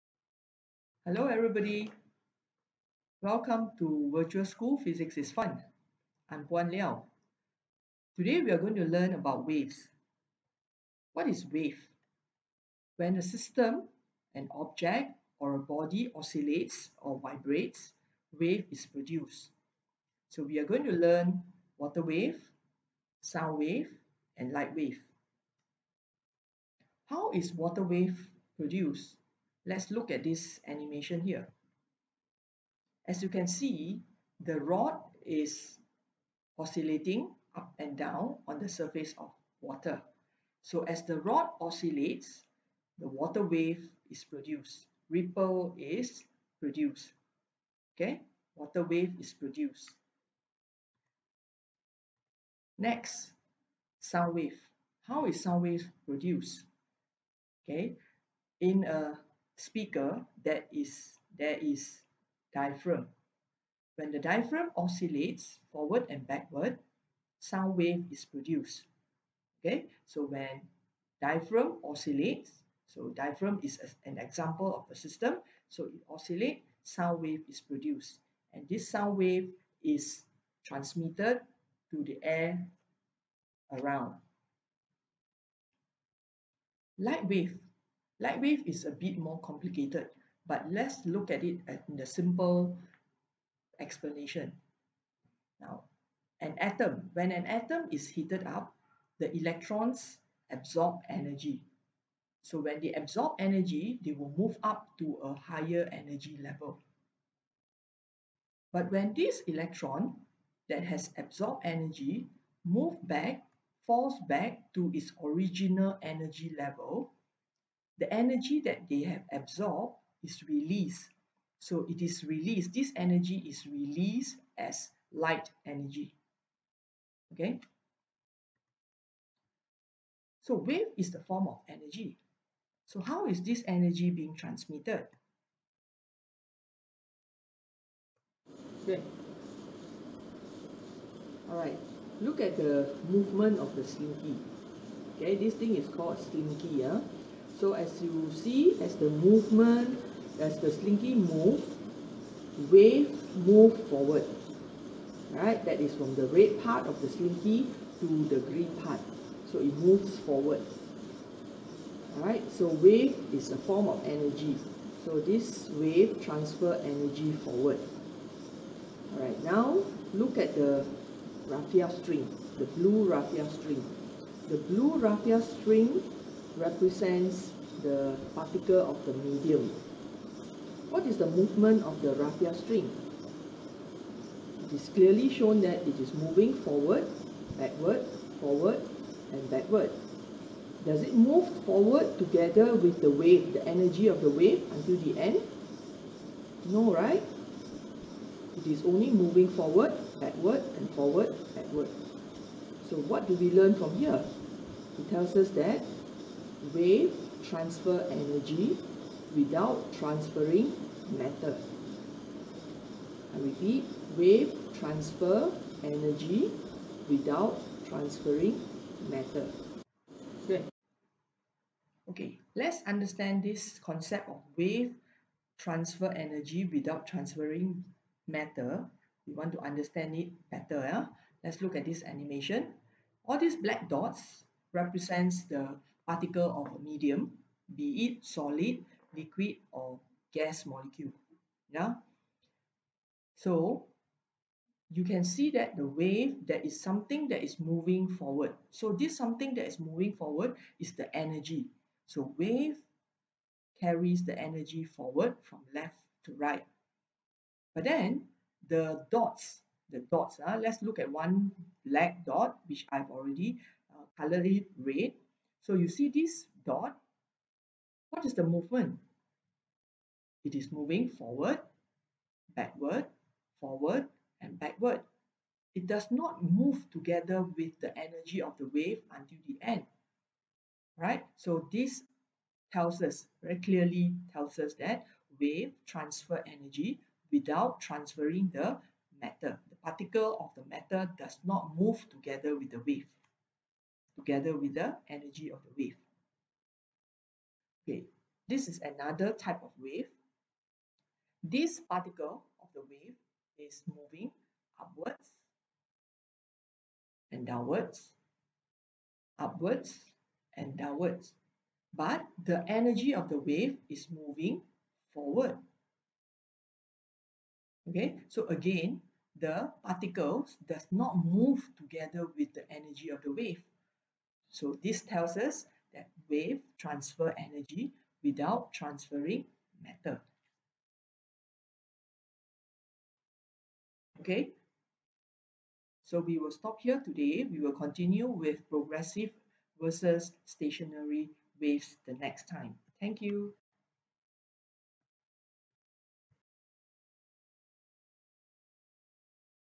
wave concept I
Wave is produced when a system vibrates. Wave transfer energy without transferring matter.